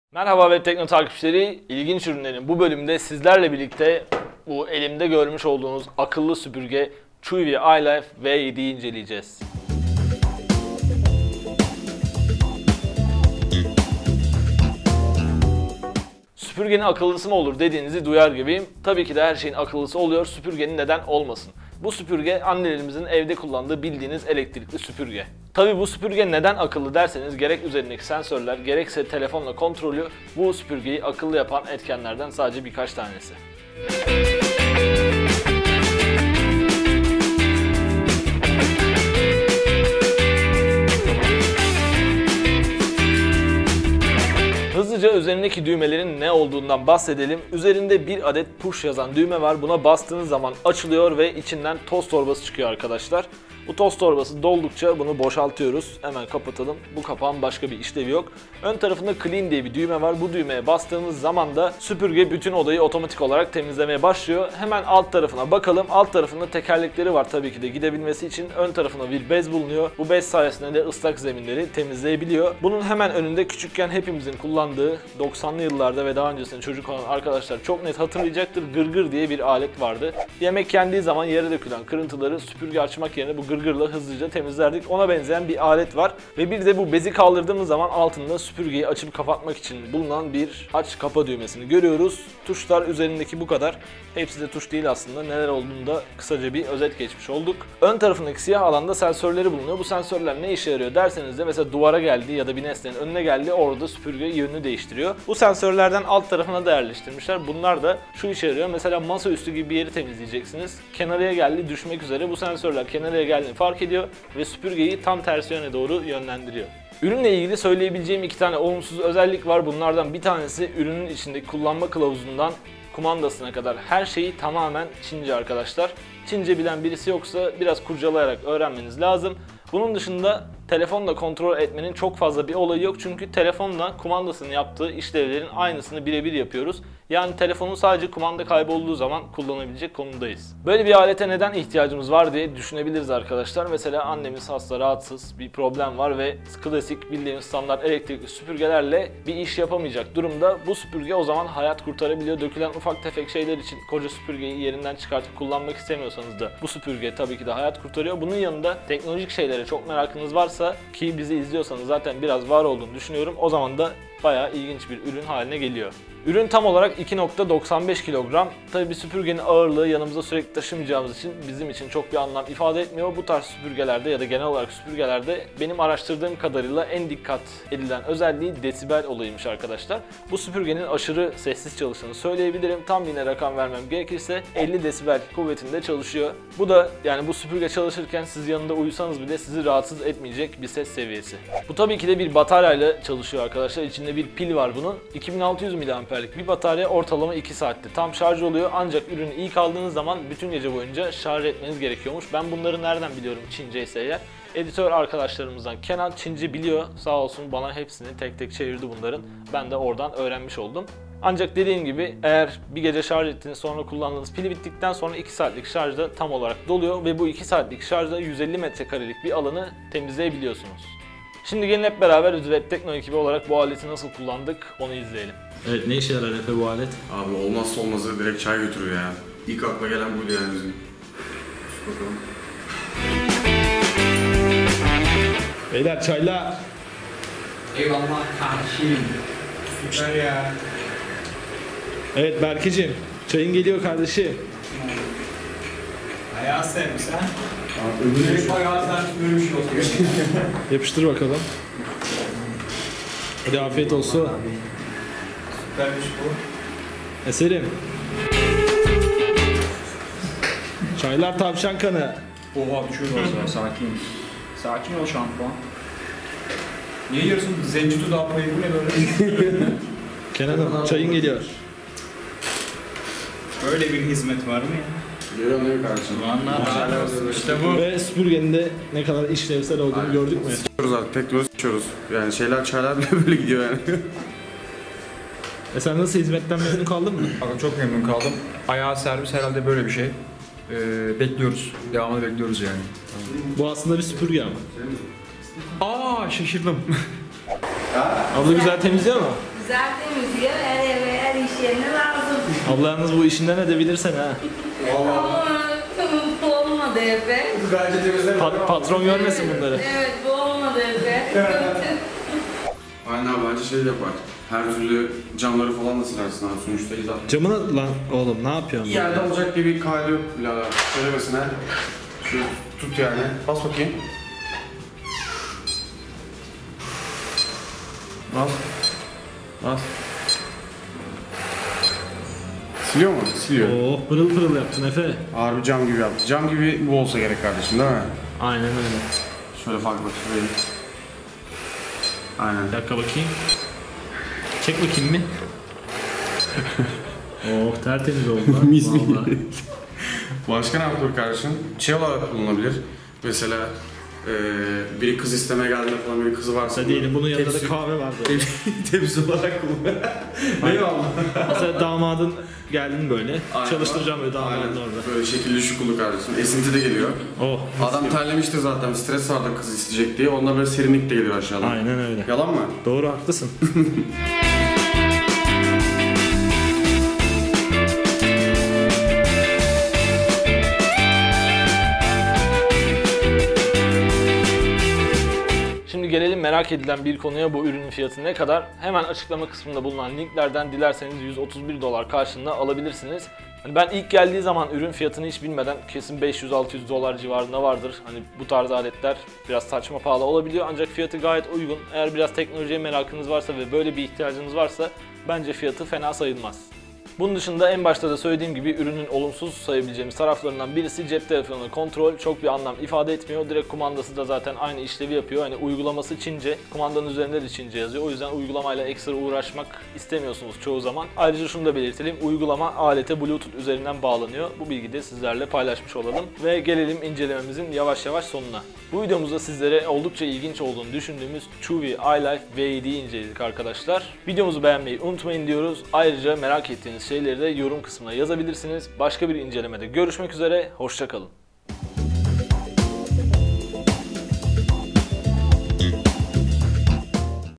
İlginç Ürünler - Akıllı Süpürge
İlginç ürünlerin bu bölümünde sizler için CHUWI ILIFE V7 Akıllı Süpürge'yi inceledik.